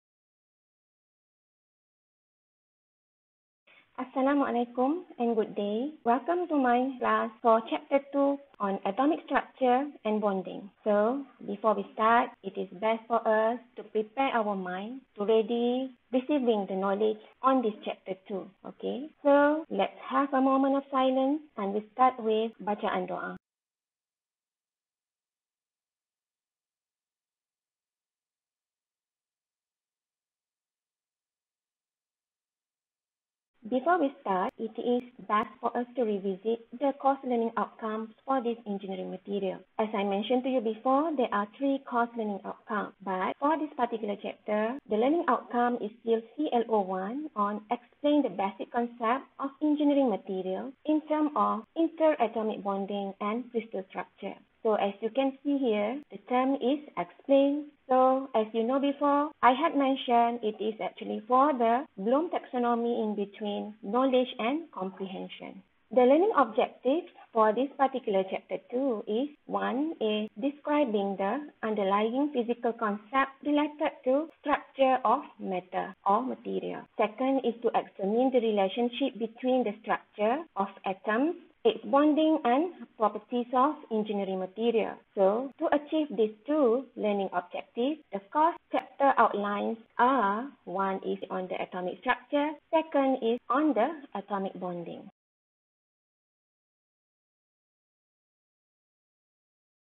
Chapter 2 Introduction to Chapter 2
This video is part of Chapter 2 of the Engineering Materials course on Atomic Structure and Bonding, taught in Universiti Teknikal Malaysia Melaka. It is for education purposes only and not for commercial use. Several related videos for Chapter 2 are: